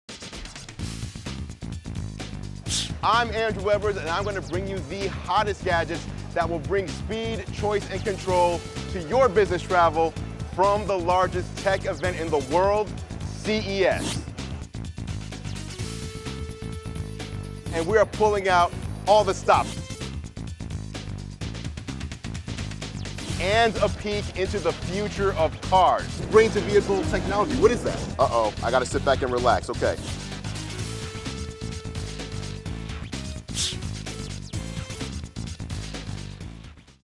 National Car Rental presents CES 2019 Live with Andru Edwards
Tune in Jan. 8 and 9 for daily live broadcasts from the show floor with tech expert Andru Edwards. Follow National on Facebook, Twitter and Instagram to catch all the action.